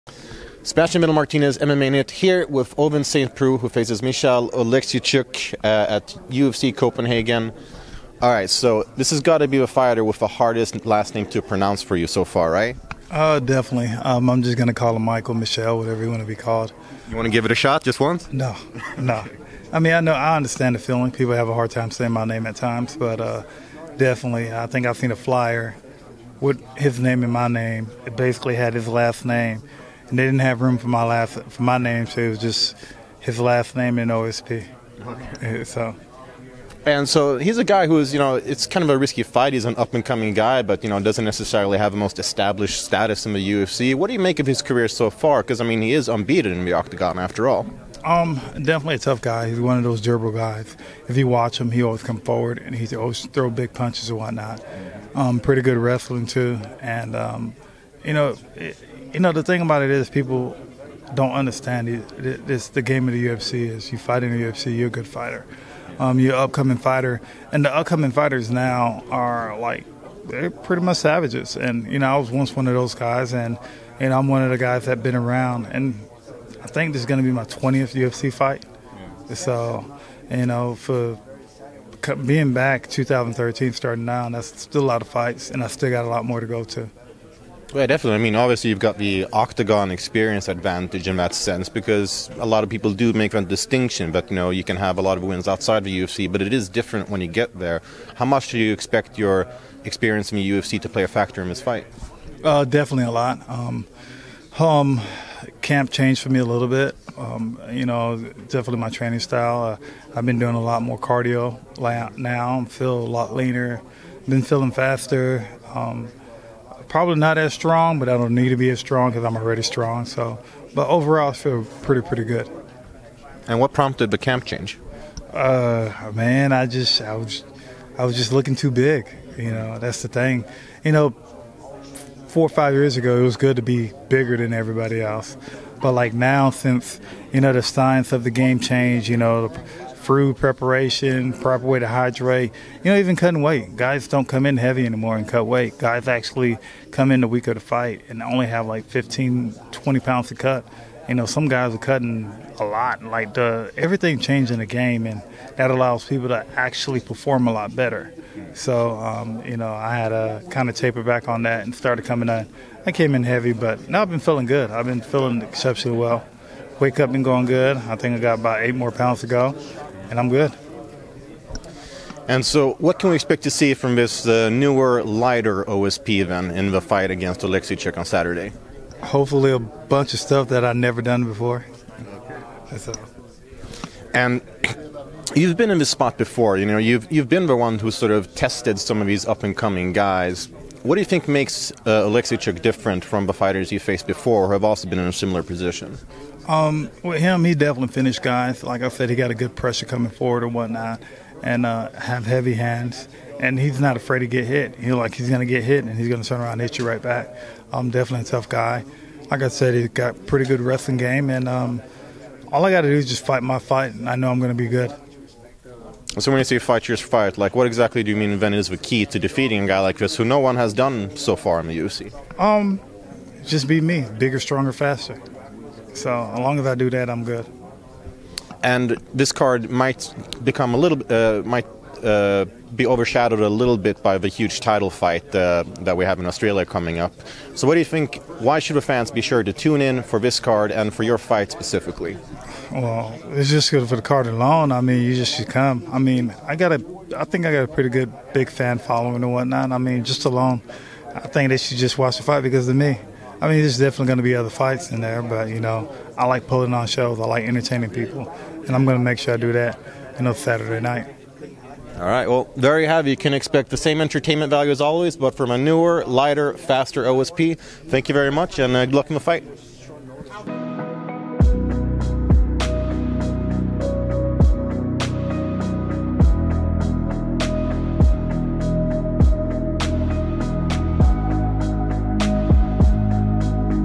Expect newer, faster Ovince Saint Preux in Denmark | UFC Copenhagen
Ovince St Preux is ready to show a new and faster 'OSP' at UFC Copenhagen against 'The Hulk' Ion Cutelaba.